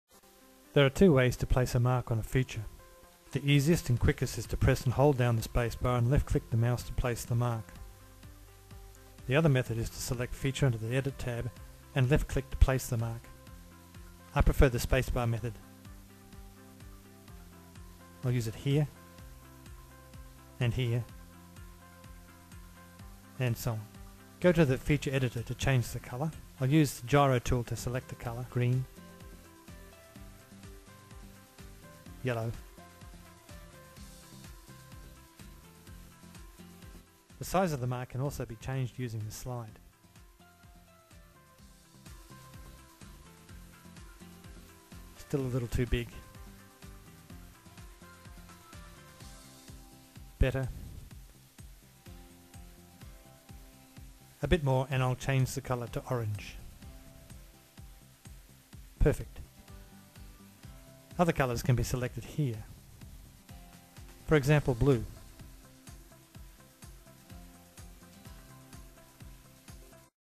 Mark up the feature - The Basics
This video shows the Fingerprint Examiner how to place marks on the image using Forensic Comparison Software.  The examiner can then to edit those marks to the color and size required.  Once set these setting remain the default until changed.